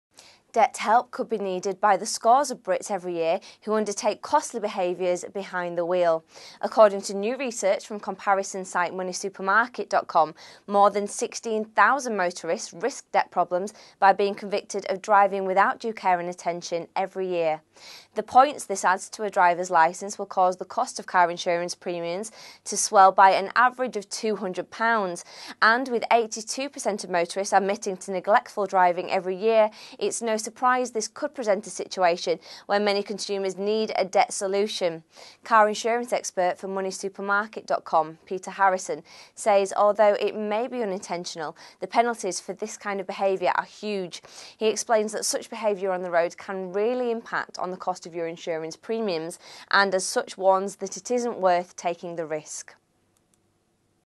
Debt help: driver error
Debt help could be needed by increasing numbers of Brits being convicted of driving without due care and attention